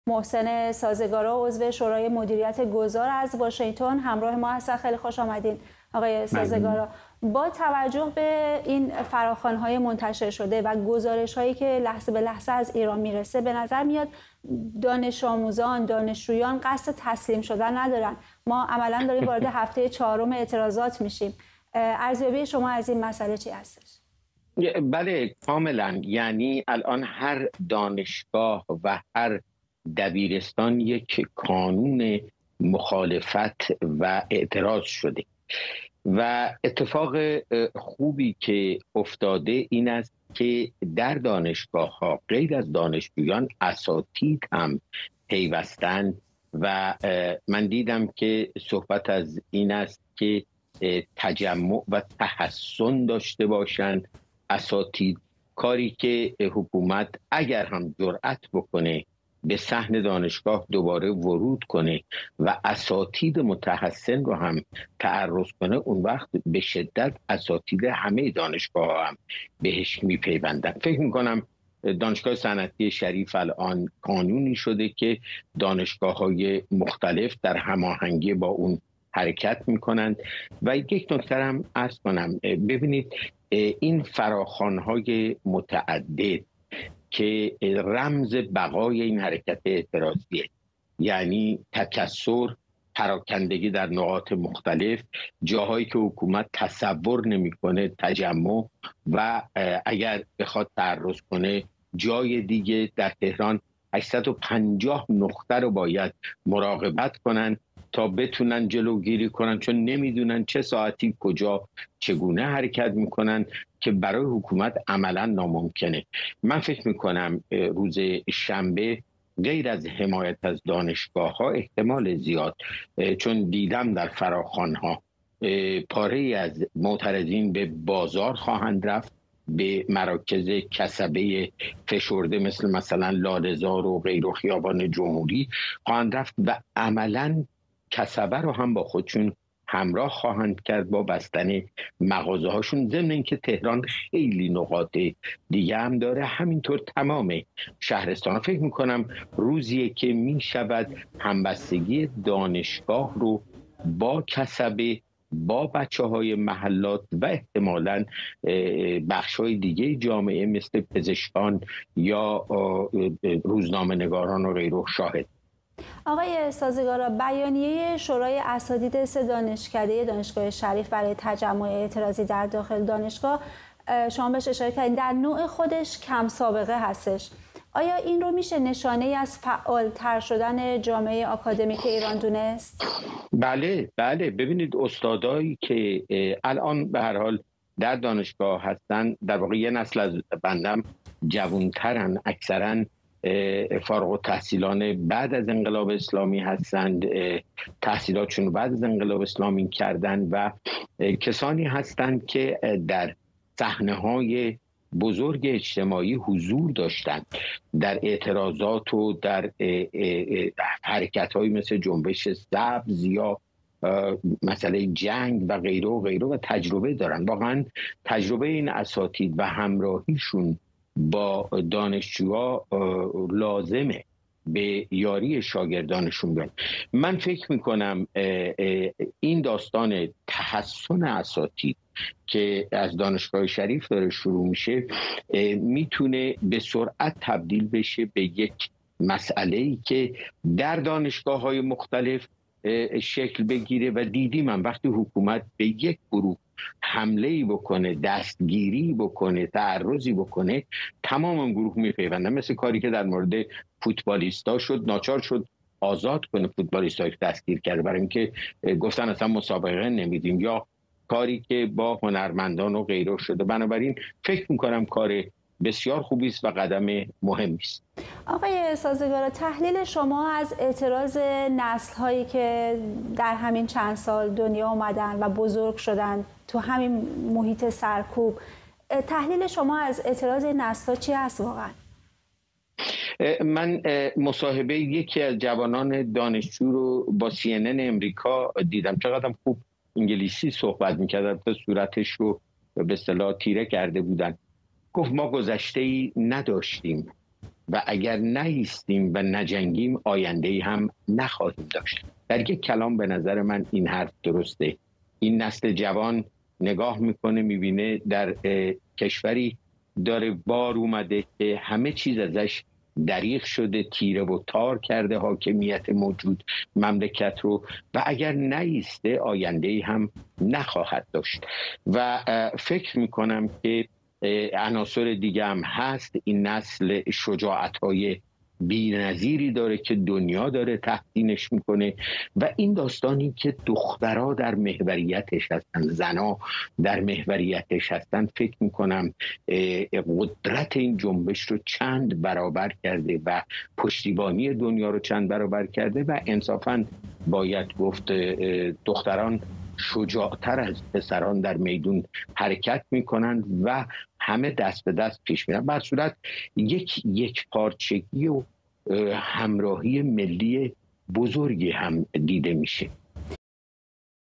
خیزش جهانی علیه جمهوری اسلامی
محسن سازگارا، عضو شورای مدیریت گذار: نسل جدید می‌بیند در کشوری دنیا آمده که حکومت همه چیز را از او دریغ کرده، پس می‌ایستد و مبارزه می‌کند تا آینده‌اش را به دست بیاورد؛ مبارزه‌ای که دنیا آن را تحسین می‌کند